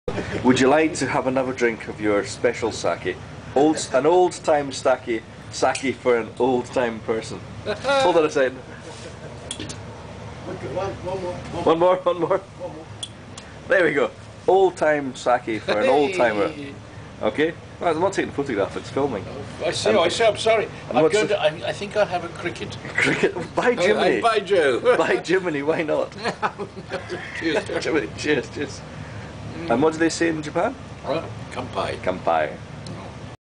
100 year old style sake OK
an amazing night in Kyotos premier Japanese-BBQ-Game-Eatanythingwitha pulsebutnotwithabackbone-style Reataurant Okariba.